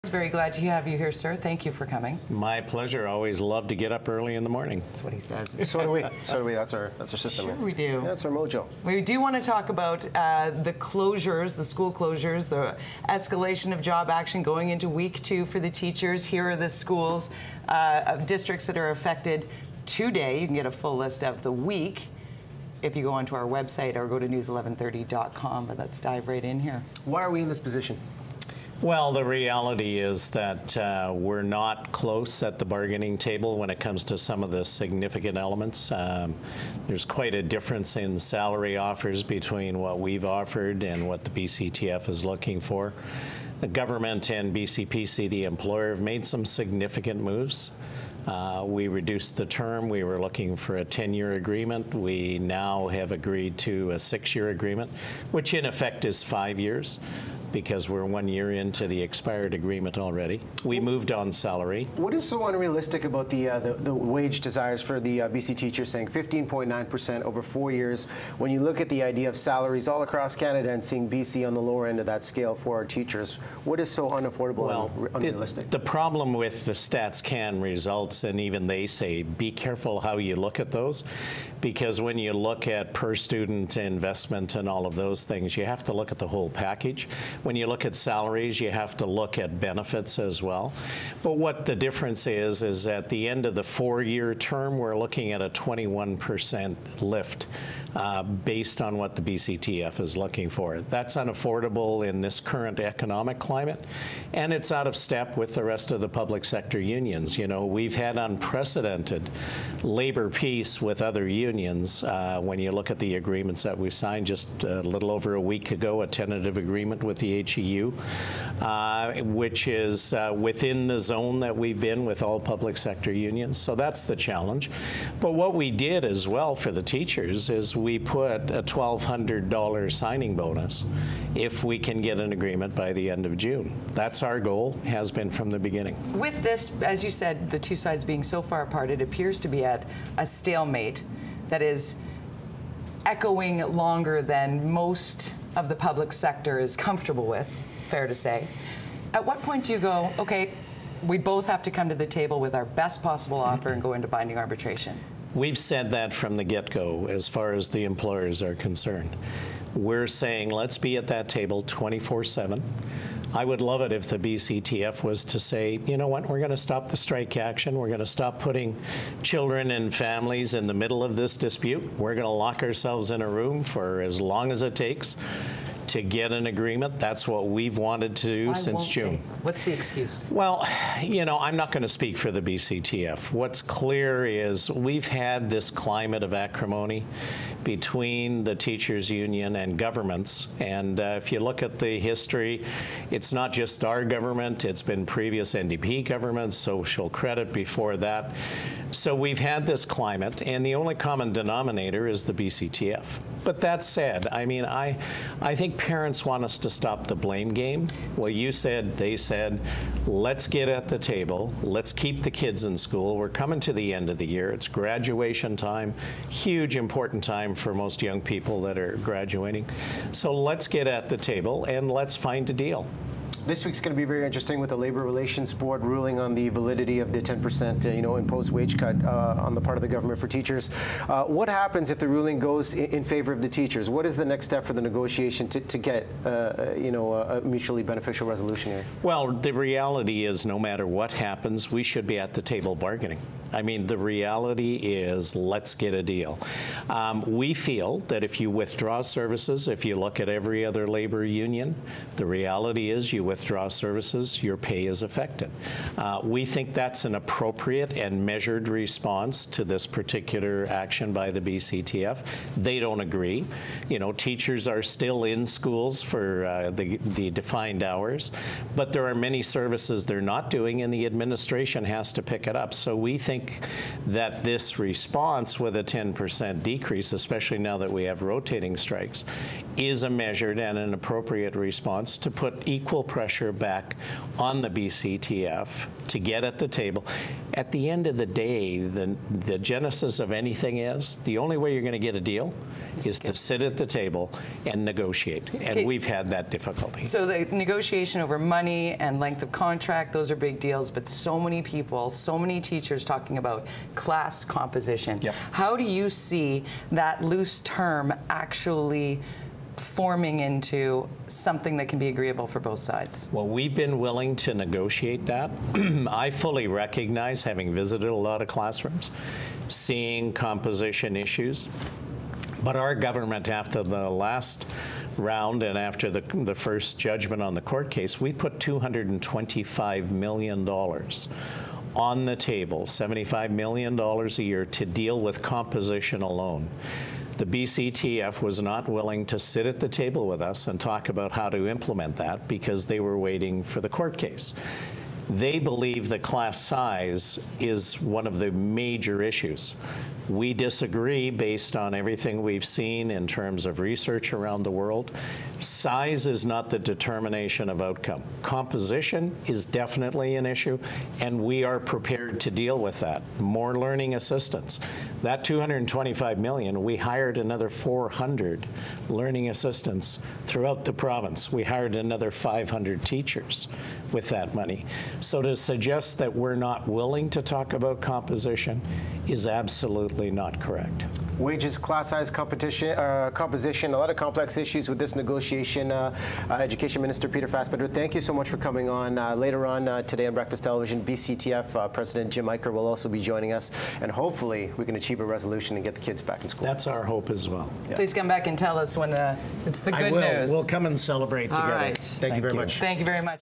Education Minister Talks Teachers
BC Education Minister Peter Fassbender joins us as the BC Teachers Federation kicks off week two of its rotating strikes across the province. We'll find out if he expects a break at the bargaining table any time soon.